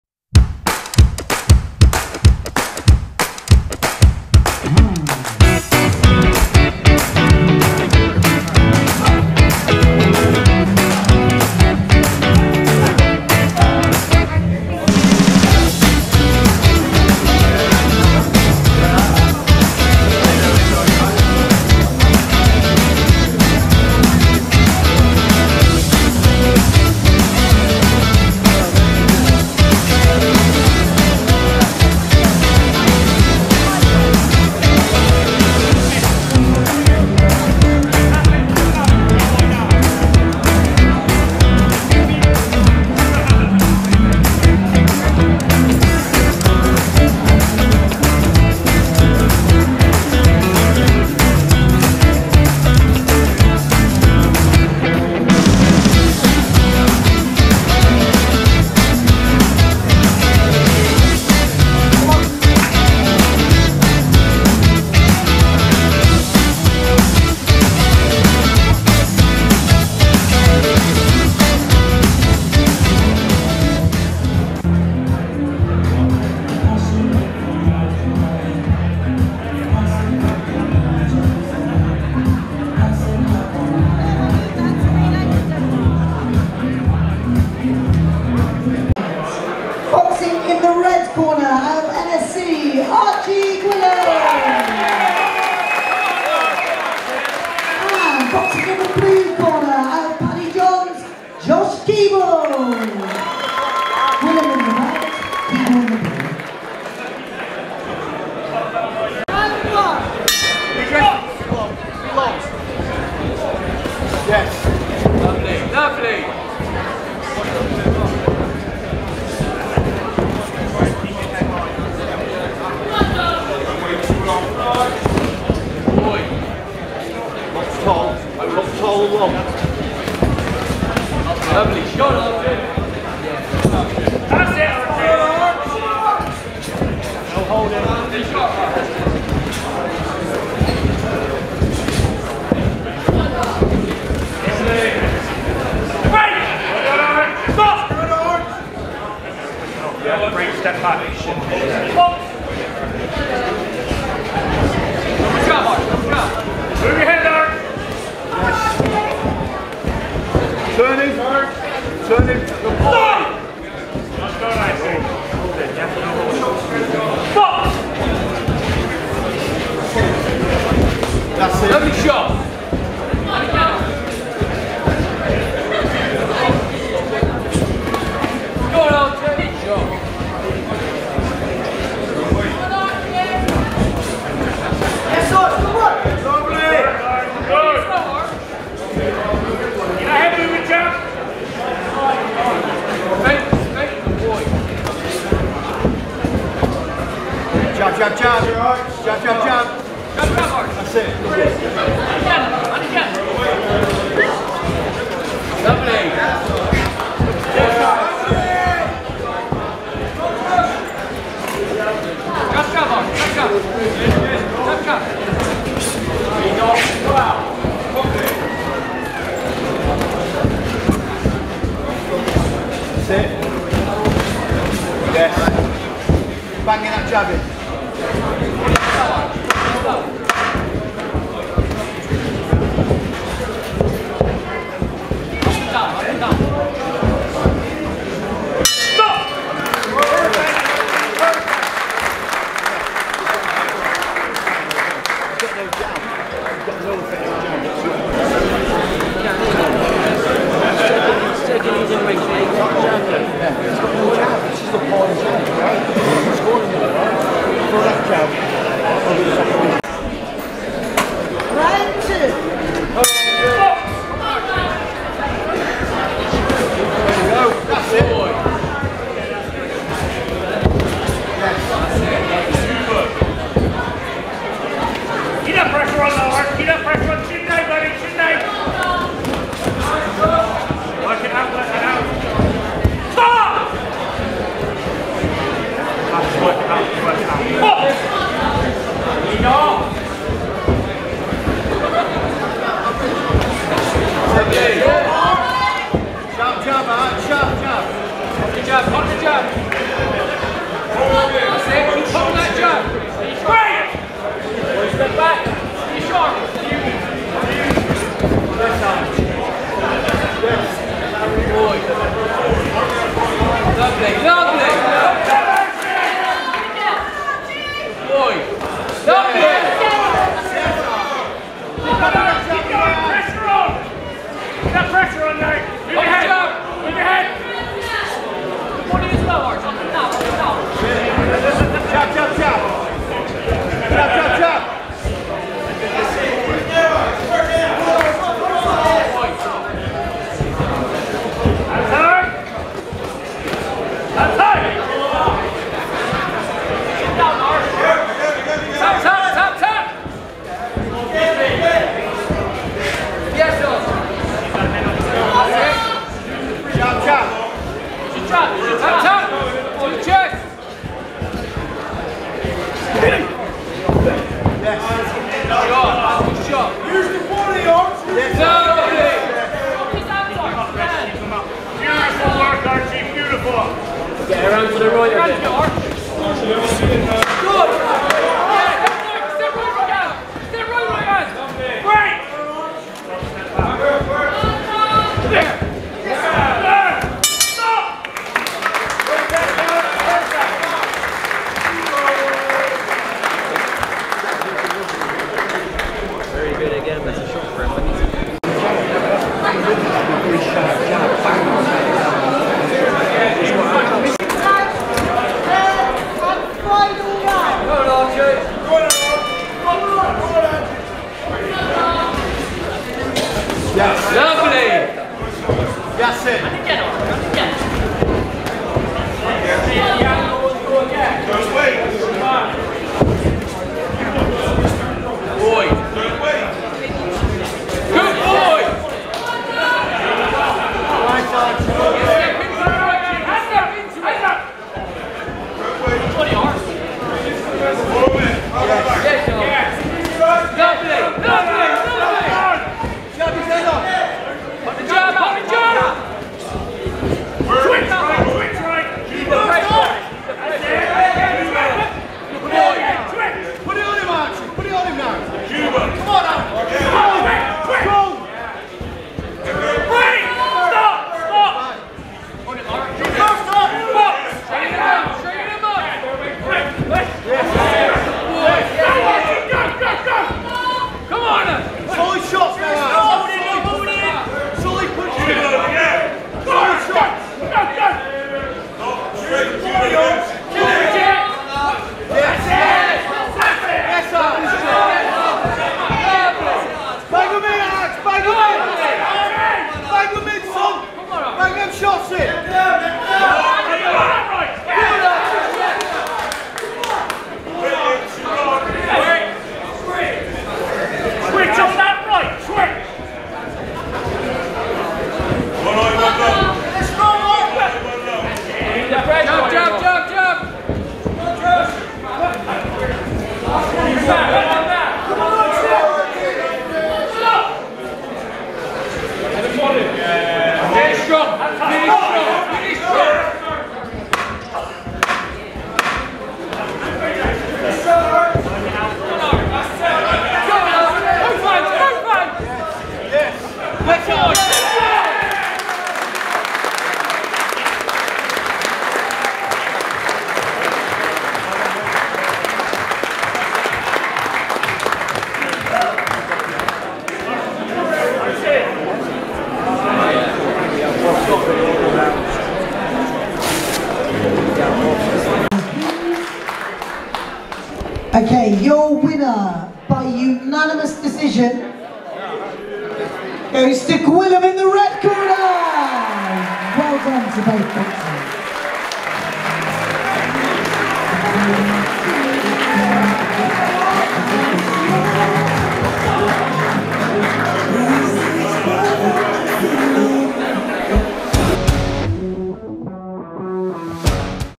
Amateur boxing at its best!
Archie Gwilyn of nsc v Josh Keeble of Paddy johns both from Bristol in this boxing match for the purest!